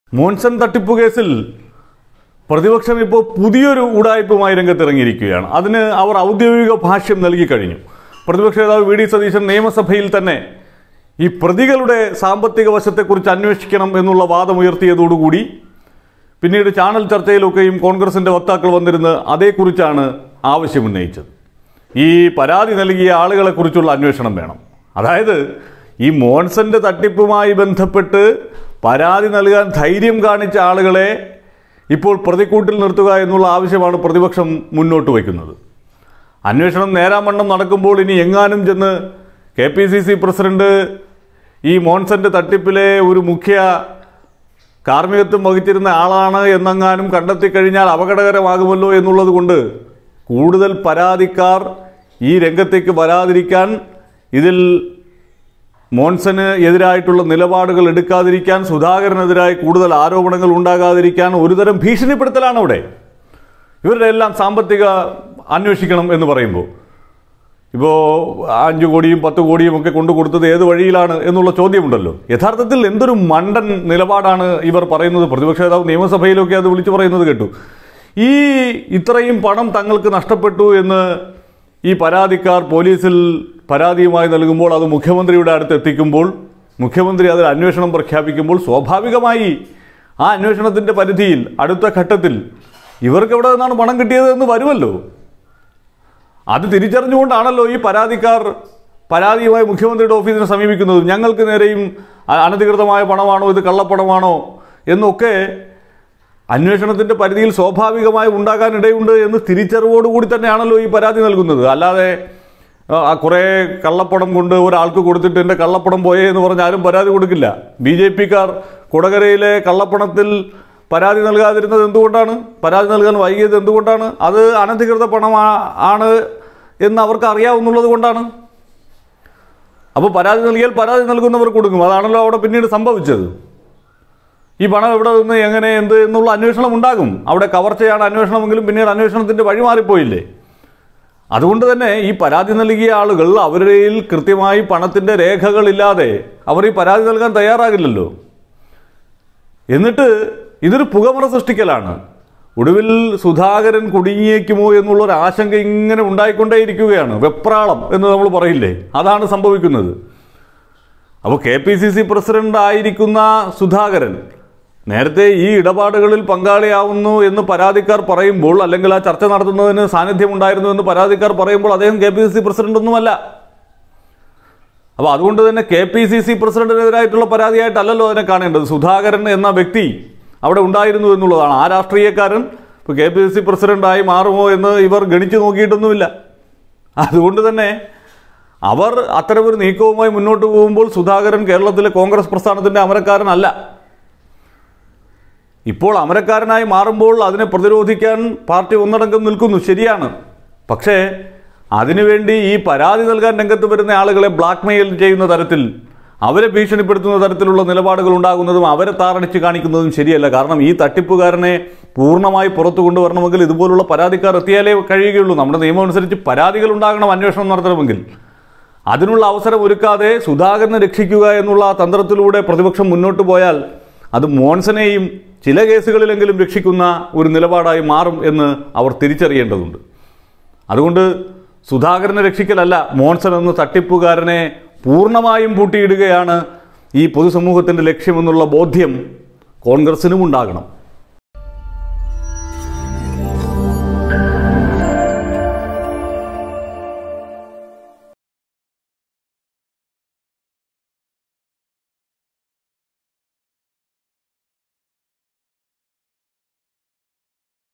മോന്‍സണെതിരായ പരാതിക്കാരെ വേട്ടയാടുന്ന പ്രതിപക്ഷം മറക്കുന്നത്.. | News Updates
പ്രതിപക്ഷം പ്ലേറ്റ് മാറ്റുന്നതെന്തിന്?
സുധാകരനെയോര്‍ത്ത് വല്ലാത്ത വെപ്രാളം

Tag-Generator-Youtube-malayalam news-global